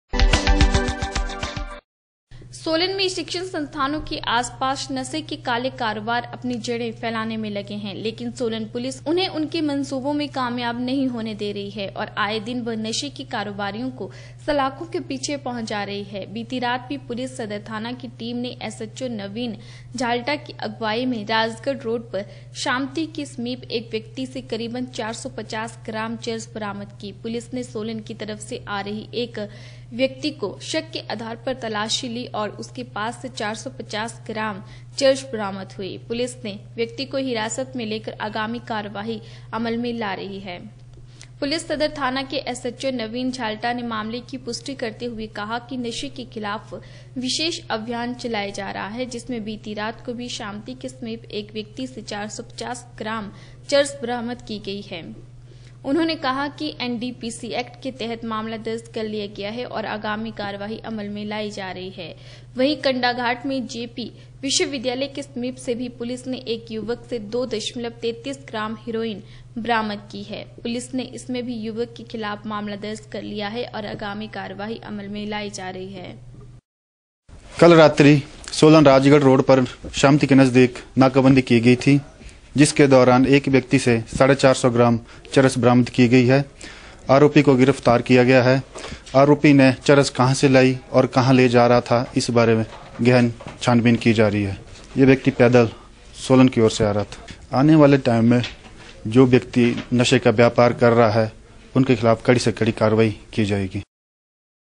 पुलिस सदर थाना की टीम ने एसएचओ नवीन झाल्टा अगवाई में एक व्यक्ति से करीब 4सौ 50 ग्राम चरस बरामद की
सोलन में शिक्षण संस्थानों के आस पास नशे के काले कारोबारी अपनी जड़ें फैलाने में लगे है । लेकिन सोलन पुलिस उन्हें उनके मनसूबों में कामयाब  नही होने दे रही है और आए दिन वह नशे के कारोबारियों को सलाखों के पीछे पहुँचा रही है । बीती रात  भी पुलिस सदर थाना की टीम ने एसएचओ नवीन झाल्टा की अगवाई में राजगढ़ रोड पर शमंती के समीप  एक व्यक्ति से करीब 4सौ 50 ग्राम चरस बरामद की  है।